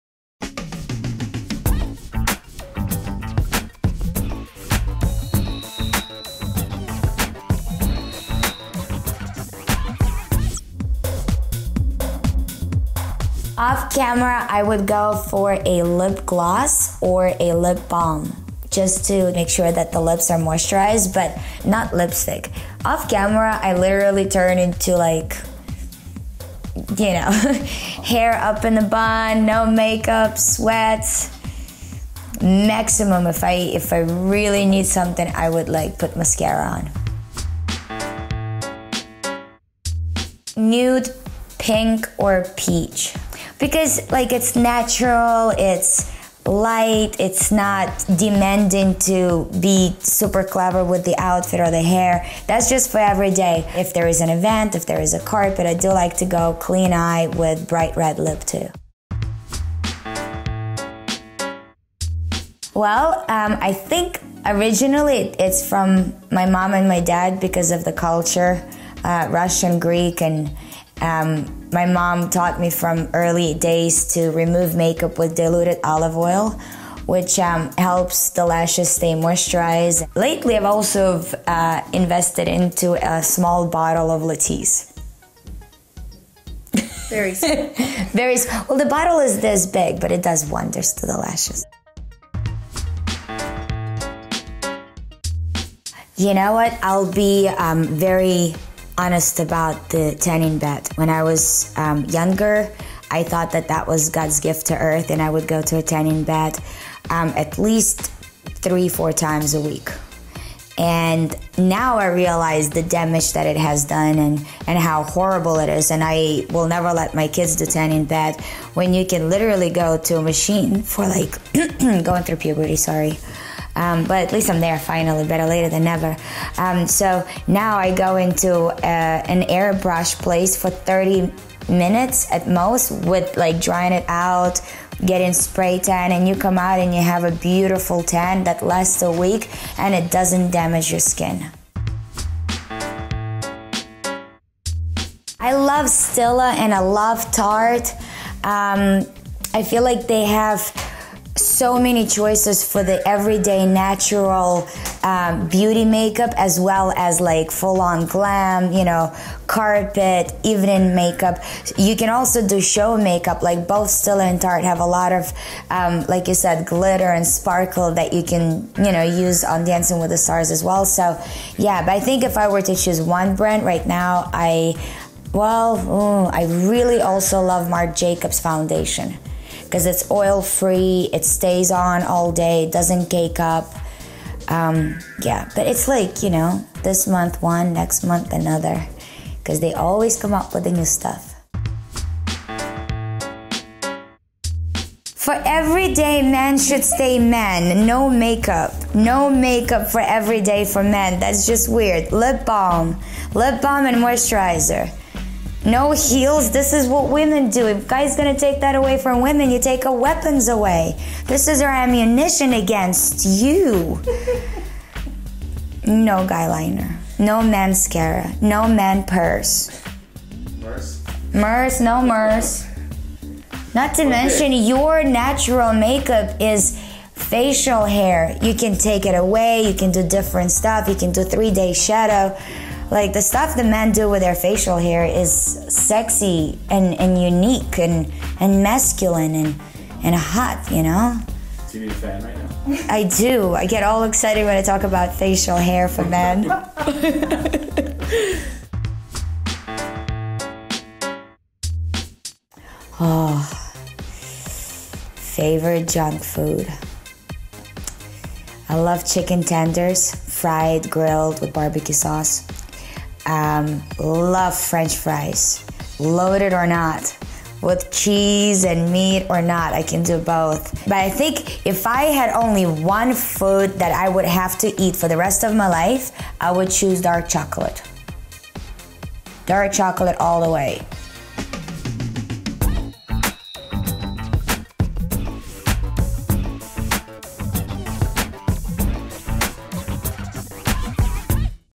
Keeping it Real w/ Karina Smirnoff
Ever wonder how Karina achieves her flawless look? Health Beauty Life was able to sneak in a few gotta-know questions while she was in makeup before her photo shoot.